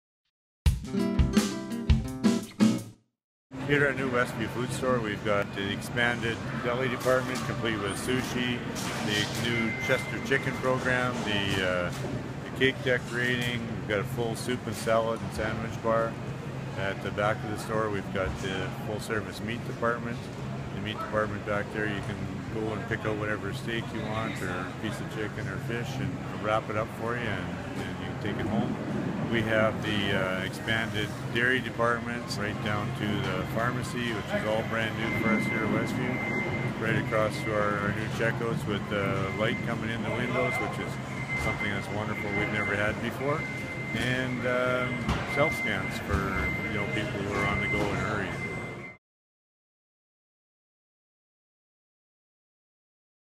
New Co-op on 33rd Street
Store manager Tim James describes some of the new features of the new Co-op store on 33rd Street that opens Friday.